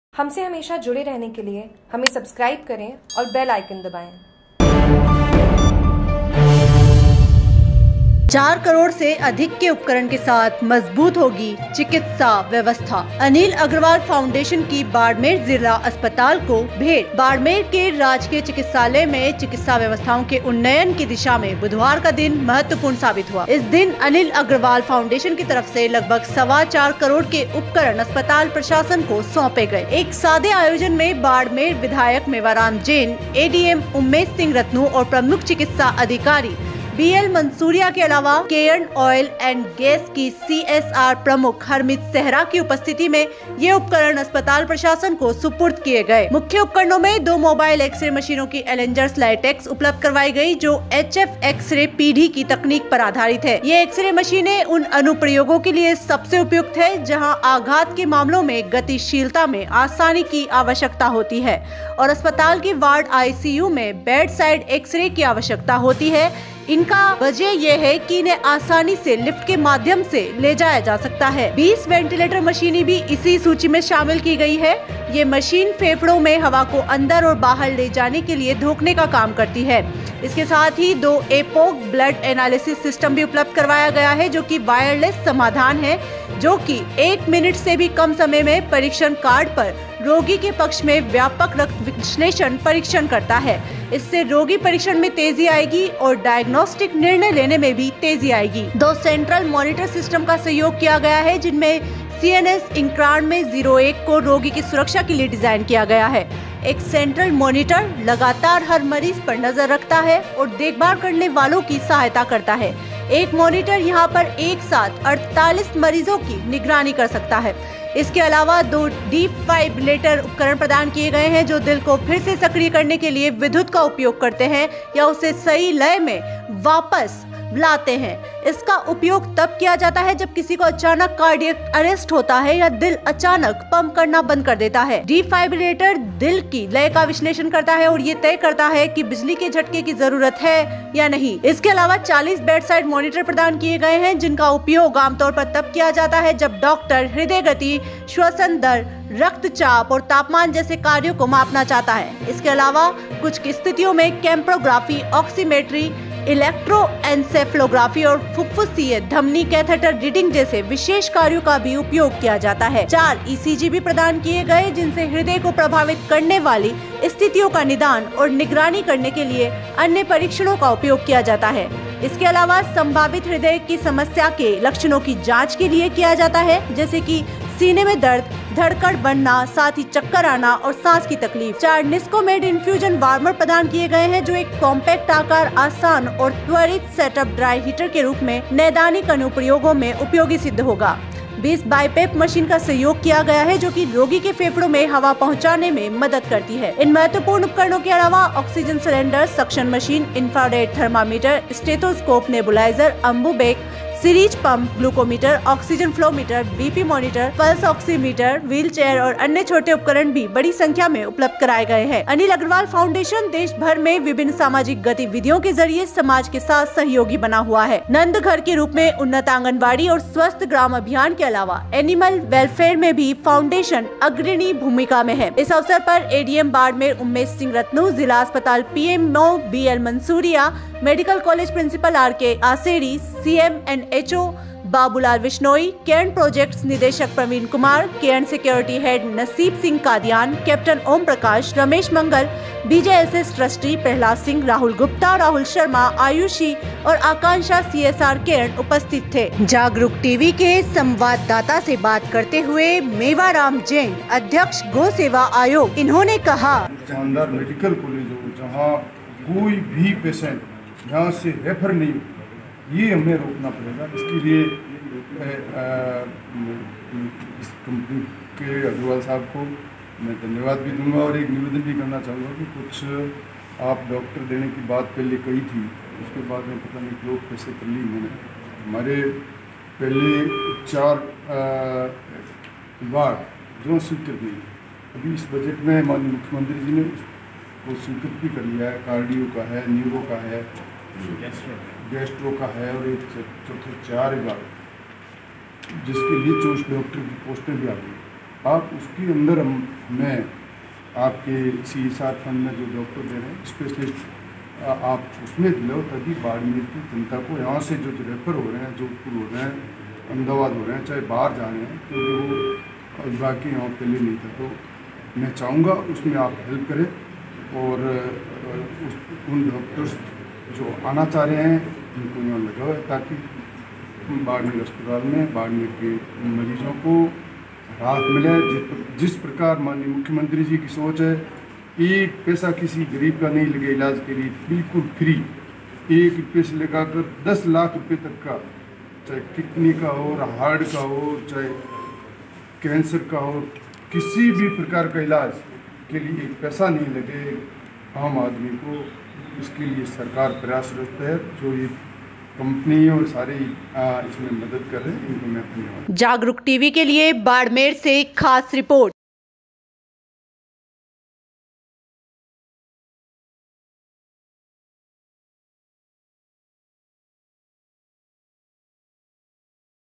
बाड़मेर : चार करोड़ से अधिक के उपकरण के साथ मजबूत होगी चिकित्सा व्यवस्था
बाड़मेर के राजकीय चिकित्सालय में चिकित्सा व्यवस्थाओं के उन्नयन की दिशा में बुधवार का दिन महत्वपूर्ण साबित हुआ। इस दिन अनिल अग्रवाल फाउंडेशन की तरफ से लगभग सवा चार करोड़ के उपकरण अस्पताल प्रशासन को सौंपे गए। एक सादे आयोजन में बाड़मेर विधायक मेवाराम जैन, ए डी एम उम्मेद सिंह रत्नू और प्रमुख चिकित्सा अधिकारी बी एल मंसुरिया के अलावा केयर्न ऑयल एंड गैस की सी एस आर प्रमुख हरमीत सेहरा की उपस्थिति में  उपकरण अस्पताल प्रशासन को सुपुर्द किए गए।

मुख्य उपकरणों में दो मोबाइल एक्स-रे मशीनों की एलेंजर्स लाइटेक्स उपलब्ध करवाई गई जो एचएफ एक्स-रे पीढ़ी की तकनीक पर आधारित है। ये एक्स-रे मशीनें उन अनुप्रयोगों के लिए सबसे उपयुक्त हैं जहां आघात के मामलों में गतिशीलता में आसानी की आवश्यकता होती है और अस्पताल के वार्ड आईसीयू में बेडसाइड एक्स-रे की आवश्यकता होती है और इसे आसानी से लिफ्ट के माध्यम से ले जाया जा सकता है।

बीस वेंटिलेटर मशीनें भी इस सूची में सम्मिलित है। यह मशीन फेफड़ों में हवा को अंदर और बाहर ले जाने के लिए धौंकनी का काम करती हैं। दो एपोक ब्लड एनालिसिस सिस्टम उपलब्ध करवाए गए हैं जो वायरलेस समाधान है जो एक मिनट से भी कम समय में परिक्षण कार्ड पर रोगी के पक्ष में व्यापक रक्त विश्लेषण परीक्षण करता है। इससे रोगी-परीक्षण में तेजी आएगी और डायग्नोस्टिक निर्णय लेने में तेजी आएगी।

दो सेंट्रल मॉनिटर सिस्टम का सहयोग किया गया है जिनमे सीएनएस-9101 को रोगी की सुरक्षा के लिए डिज़ाइन किया गया है। एक सेंट्रल मॉनिटर लगातार हर मरीज पर नजर रखता है और देखभाल करने वालों की सहायता करता है। एक मॉनिटर एक साथ 48 मरीजों की निगरानी कर सकता है।

इनके अलावा दो डिफाइब्रिलेटर उपकरण प्रदान किए गए हैं जो दिल को फिर से सक्रिय करने के लिए विद्युत का उपयोग करता है या उसे सही लय में वापस लाता है। इसका उपयोग तब किया जाता है जब किसी को अचानक कार्डियक अरेस्ट होता है या जब दिल अचानक पंप करना बंद कर देता है। डिफाइब्रिलेटर दिल की लय का विश्लेषण करता है और यह तय करता है कि बिजली के झटके की जरूरत है या नहीं।

इसके अलावा चालीस बेड साइड मॉनिटर प्रदान किए गए हैं जिनका उपयोग आमतौर पर तब किया जाता है जब डॉक्टर हृदय गति, श्वसन दर, रक्तचाप और तापमान जैसे कार्यों को मापना चाहता है। इसके अलावा, कुछ स्थितियों में कैप्नोग्राफी, ऑक्सीमेट्री, इलेक्ट्रोएन्सेफलोग्राफी और फुफ्फुसीय धमनी कैथेटर रीडिंग जैसे विशेष कार्यों का भी उपयोग किया जाता है।

चार ईसीजी प्रदान किए गए जिनसे हृदय को प्रभावित करने वाली स्थितियों का निदान और निगरानी करने के लिए अन्य परीक्षणों के लिए उपयोग किया जाता है। इसका उपयोग संभावित हृदय समस्या के लक्षणों की जांच के लिए किया जा सकता है, जैसे सीने में दर्द, धड़कन बढ़ना, चक्कर आना और सांस की तकलीफ। चार निस्कोमेड इन्फ्यूजन वार्मर प्रदान किए गए हैं जो एक कॉम्पैक्ट आकार, आसान और त्वरित सेट अप ड्राई हीटर के रूप में नैदानिक अनुप्रयोगों में उपयोगी सिद्ध होगा। बीस बायीपेप मशीन का सहयोग किया गया है जो रोगी के फेफड़ों में हवा पहुंचाने में मदद करती है।

इन महत्वपूर्ण उपकरणों के अलावा ऑक्सीजन सिलेंडर, सक्शन मशीन, इन्फ्रारेड थर्मामीटर, स्टेथोस्कोप, नेबुलाइजर, अंबु बैग, सिरिंज पंप, ग्लूकोमीटर, ऑक्सीजन फ्लो मीटर, बीपी मॉनिटर, पल्स ऑक्सीमीटर, व्हील चेयर और अन्य छोटे उपकरण भी बड़ी संख्या में उपलब्ध कराए गए हैं। अनिल अग्रवाल फॉउंडेशन देश भर में विभिन्न सामाजिक गतिविधियों के जरिए समाज के साथ सहयोगी बना हुआ है। नन्द घर के रूप में उन्नत आंगनवाड़ी और स्वस्थ ग्राम अभियान के अलावा एनिमल वेलफेयर में भी फाउंडेशन अग्रणी भूमिका में है।

इस अवसर पर  एडीएम बाड़मेर उम्मेद सिंह रत्नू,  जिला अस्पताल पीएमओ बी एल मंसुरिया, मेडिकल कॉलेज प्रिंसिपल आरके आसेरी , सीएम एंड एचओ बाबू लाल विश्नोई ,  केयर्न प्रोजेक्ट्स  निदेशक  प्रवीण कुमार,   केयर्न सिक्योरिटी हेड नसीब सिंह कादियान, कैप्टन ओम प्रकाश, रमेश मंगल बीजेएसएस ट्रस्टी, प्रहलाद सिंह, राहुल गुप्ता, राहुल शर्मा, आयुषी और आकांक्षा सीएसआर केयर्न उपस्थित थे।